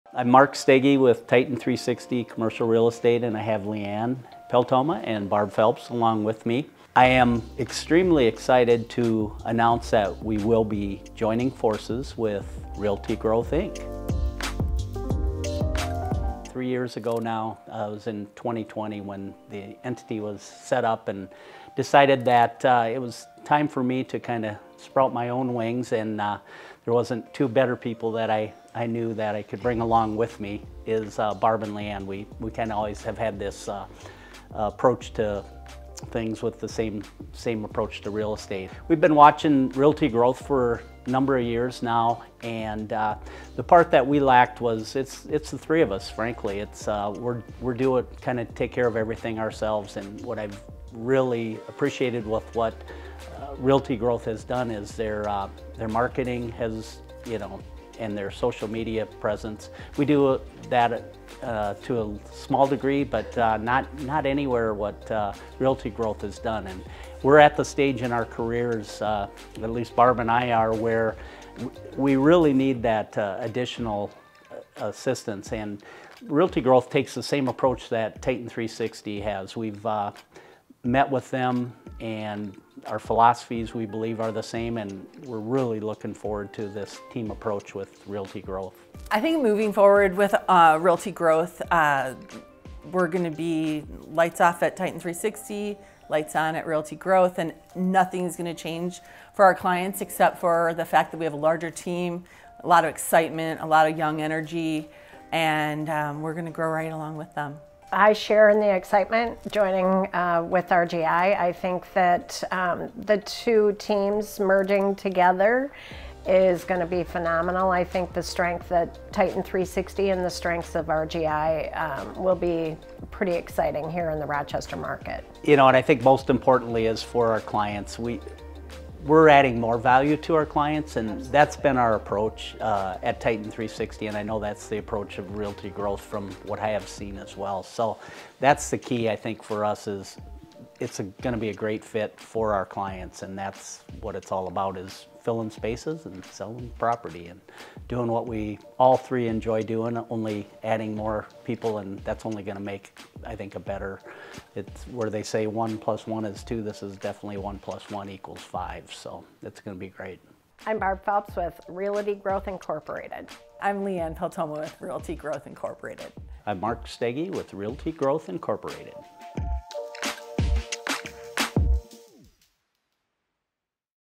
Titan 360 is joining forces with RGI!
We would like to welcome our new team members!

In this video you will hear from Barb Phelps, Leigh Ann Peltoma, and Mark Steege. These real estate veterans have helped many clients over the years and have a depth of knowledge when it comes to Commercial Real Estate. We are extremely excited to share that all three individuals have joined our RGI team to help us continue the legacy of providing superior service to our clients.

We can not wait to bring you along for the journey as Barb, Leigh Ann, and Mark assist us in growing this great company.

Contact our office today for a tour or more information!